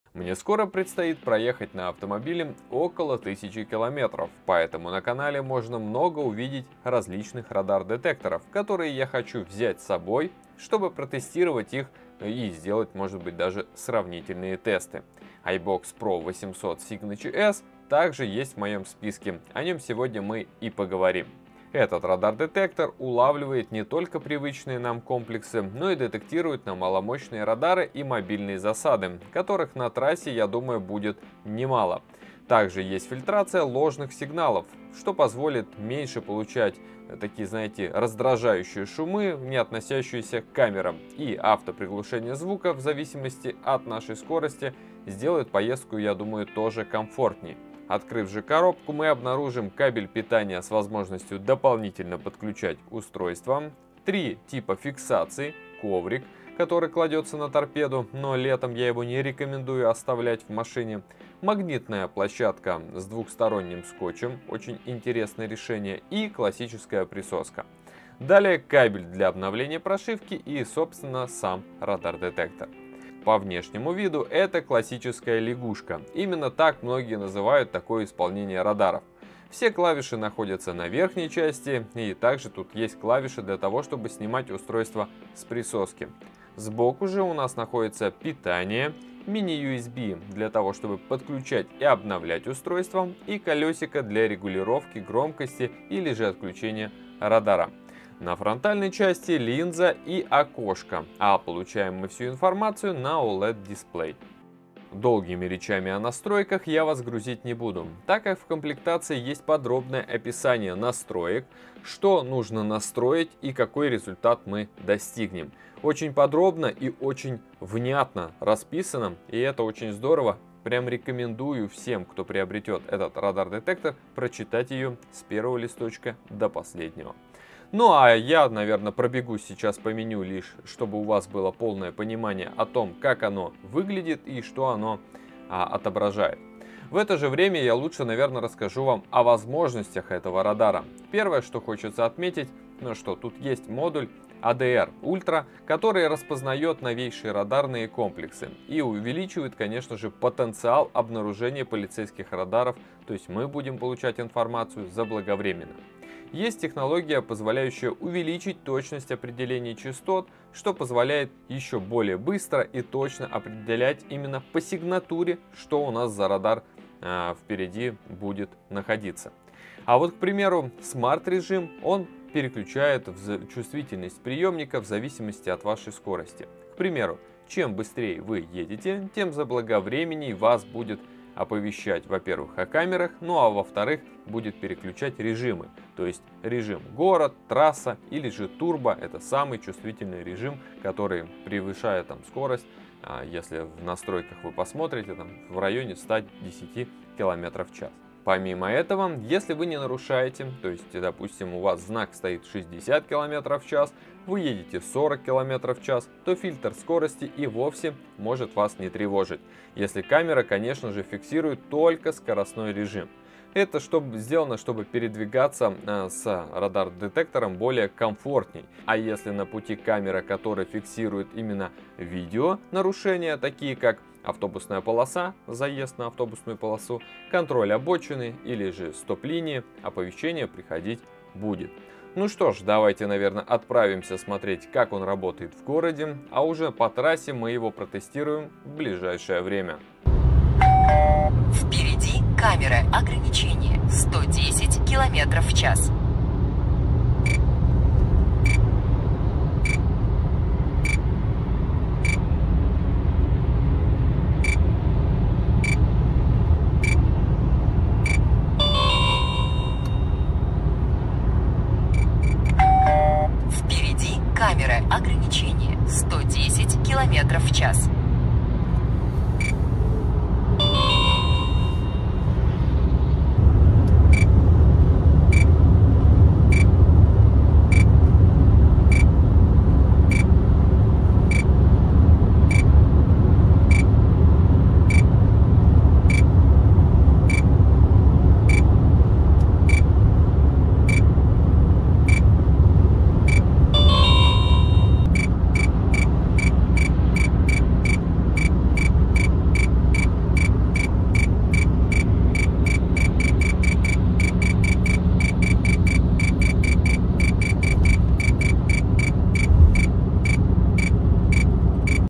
iBOX Pro 800 Signature X - Первое впечатление от сигнатурного радар детектора. Нужен тест на трассе
iBOX Pro 800 Signature X - радар-детектор с GPS/ГЛОНАСС базой камер, оснащенный улучшенным сверхчувствительным модулем ADR Ultra для максимальной дальности обнаружения полицейских камер, совместно с режимом Smart, значительно увеличивает потенциал радар-детектора в дальности обнаружения полицейских радаров, в т.ч. маломощных радаров при детектировании «в спину». Радиомодуль ibox способен детектировать радары Робот, Стрелка, Кордон, Крис, Крис-П, Искра, Радис, Арена, Бинар, Беркут, Сокол, ВКС, Барьер-2М, ПКС-4 и Визир и др. Высокочувствительный лазерный модуль обеспечивает возможность обнаружения лазерных сигналов на 360° и позволяет обнаружить такие радары, как ПОЛИСКАН, АМАТА, ЛИСД, ЛИСД 2. В устройстве установлен радарный модуль второго поколения на базе высокопроизводительного процессора ST Microelectronics с технологией Signature Sensitivity Platform (SSP) увеличивающий чувствительность и дальность обнаружения. Фильтр X Signature автоматически выключает звуковые оповещения о радаре при силе сигнала ниже установленного значения. GPS-модуль с обновляемой базой данных камер 45-ти стран (Россия, СНГ: Узбекистан, Казахстан и др.), оповещает о «малошумных» камерах (Кордон, Кречет, Mesta, Рапира, Вокорд «Циклоп», RedSpeed, Скат, Робот, Multaradar, Одиссей), и «парных камерах» без радарного блока, вычисляющих среднюю скорость (Автоураган, Автодория, Сергек).

00:00 ► Вступление
00:21 ► Коротко про iBOX Pro 800 Signature X
00:50 ► Комплектация
01:13 ► Обзор радар детектора
01:46 ► Изучение настроек
04:00 ► Первые тесты

Ну что айбошим_с_iBOX ?)